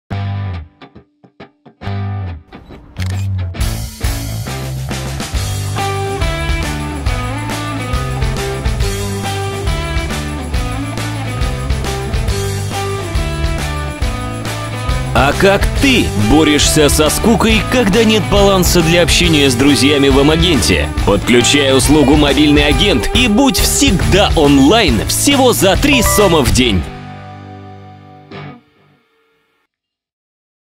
"Мобильный Агент" от сотового оператора Beeline
Рекламный ролик по продвижению услуги "Мобильный Агент" от сотового оператора Beeline.
Клиент: Beeline
Продакшн: Antares Creative Group